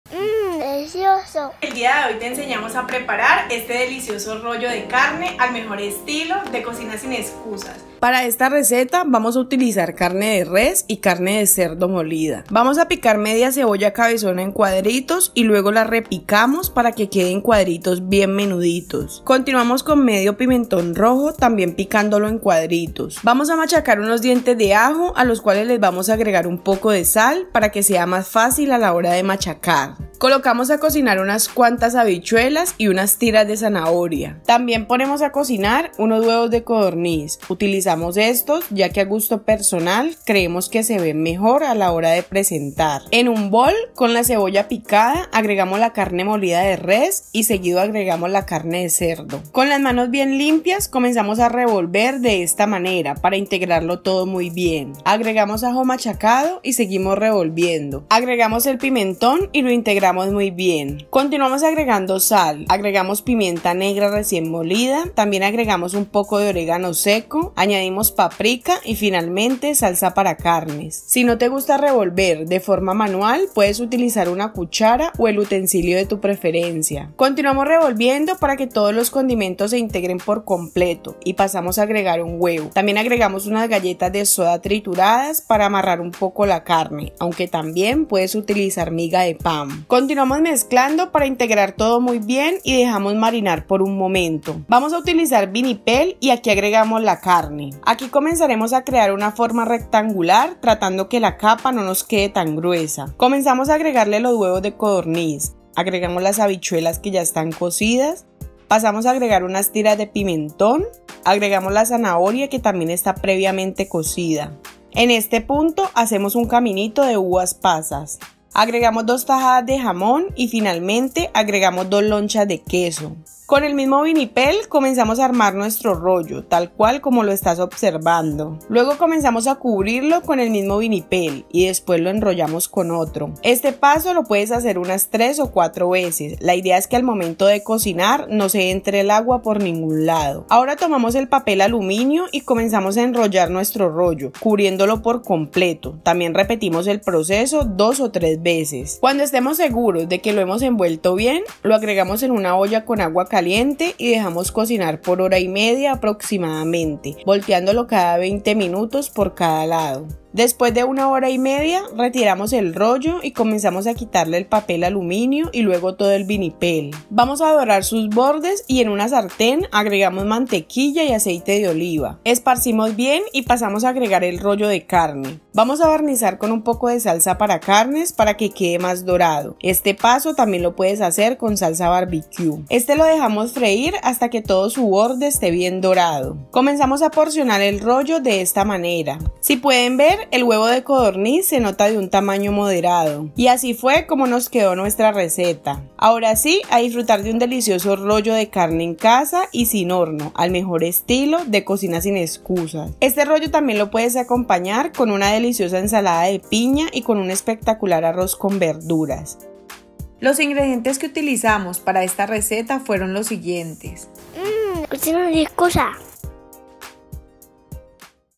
Cómo hacer Rollo de Carne super Fácil
La Receta del día de hoy es un Delicioso Rollo de Carne sin Horno Super Fácil al mejor estilo de Cocina Sin Excusas

Los INGREDIENTES Son:
-✅Carne de res y cerdo
-✅Zanahoria
-✅Habichuelas
-✅Pimentón
-✅Uvas pasas

-✅Huevos
-✅Jamón

-✅Queso
-✅Cebolla
-✅Pimienta
-✅Sal

-✅Orégano

-✅Paprika
-✅Salsa para carnes

-✅Ajo